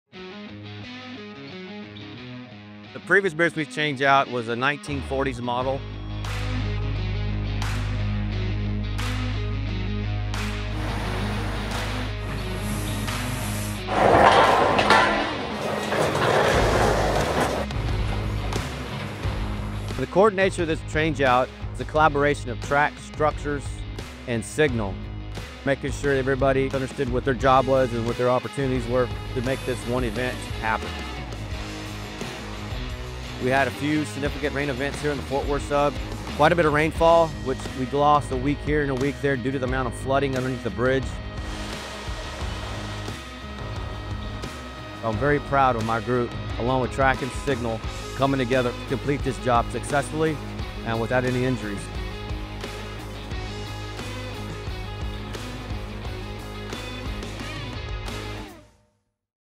BNSF teams replace bridge in just 28 hours!
In just 28 hours, BNSF crews safely demolished an 85-year-old wooden trestle bridge and built its 600-foot steel-and-concrete replacement north of Justin, Texas. More than 40 team members worked around the clock to ensure the new structure was ready for service. After months of planning, teamwork and engineering precision, everything came together in a tightly choreographed operation that showcased the pride of railroading.

You can also see a lightning-fast timelapse video of 28 hours of work condensed to one minute here on our YouTube channel.